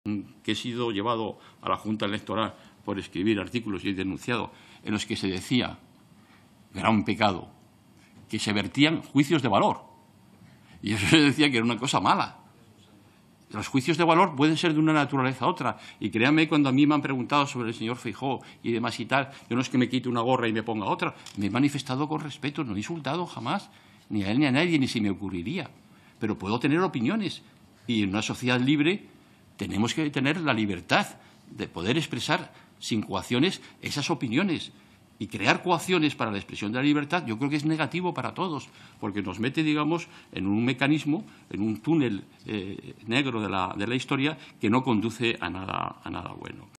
Tezanos defiende su derecho a expresar libremente su opinión sobre Feijóo
El presidente del Centro de Investigaciones Sociológicas (CIS), José Félix Tezanos, ha defendido este miércoles su derecho a expresar "libremente y sin coacciones" su opinión sobre el líder del PP, Alberto Núñez Feijóo, al que, según ha recalcado, siempre ha tratado "con respeto" y "jamás" ha "insultado".

(Fuente: Congreso)